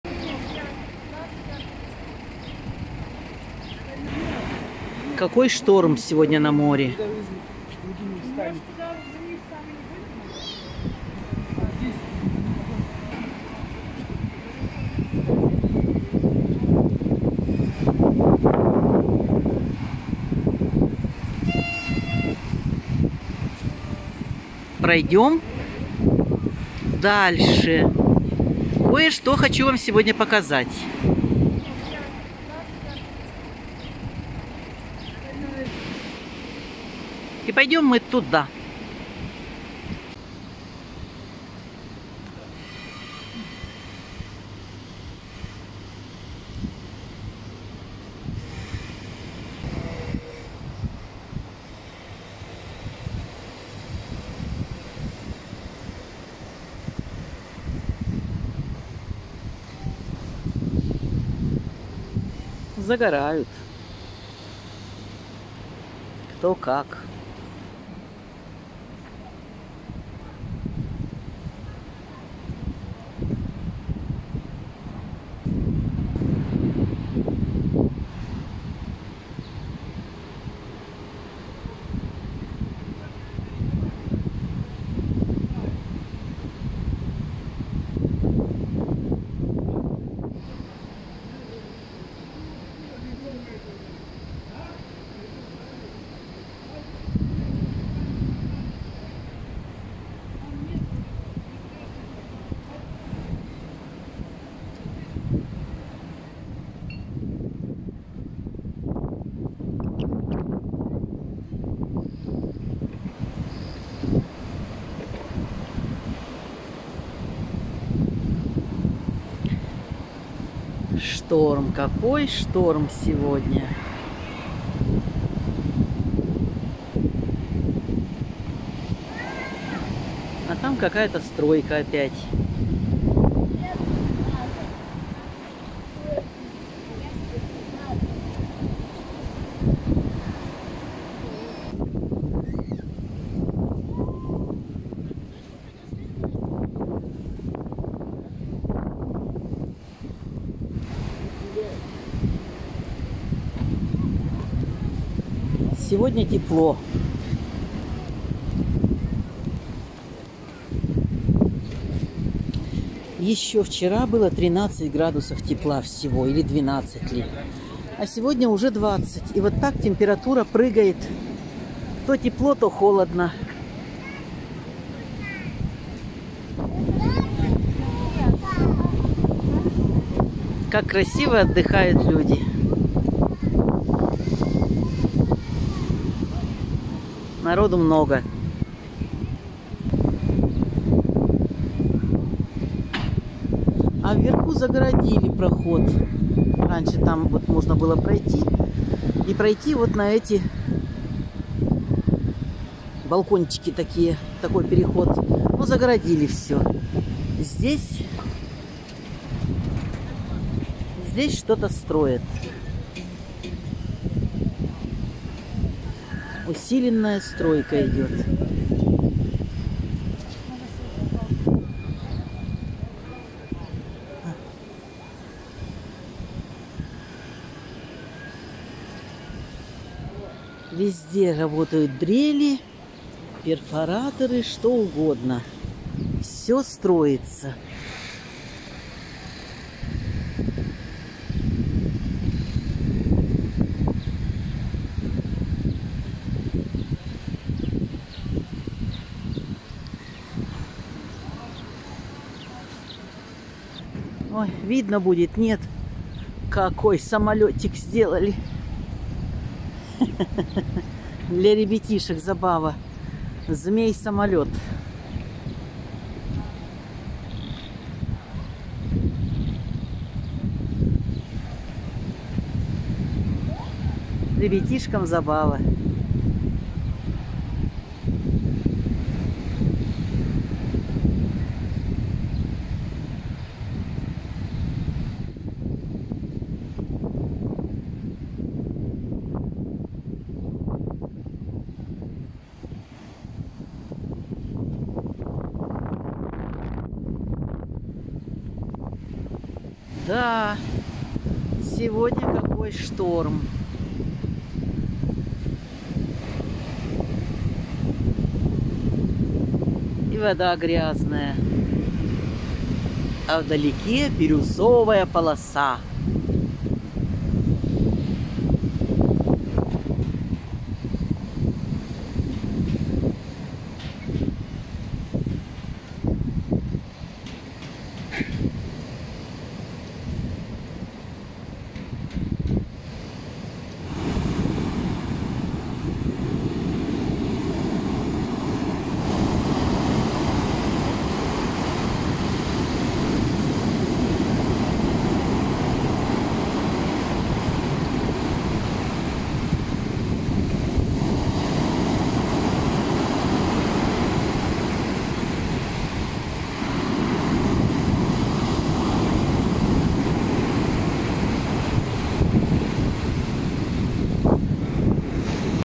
Шторм на пляже Ривьера в Сочи
Пляж "Ривьера" сегодня. На море шторм. Черное море разгулялось. В Сочи сегодня теплая, но ветренная погода. Солнышко светит, люди гуляют, море бушует. Все, что надо для отдыха.
Друзья!  График выхода роликов ежедневно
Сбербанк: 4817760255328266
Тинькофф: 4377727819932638
Буду очень благодарна Вам, если поделитесь этим видео со своими друзьями!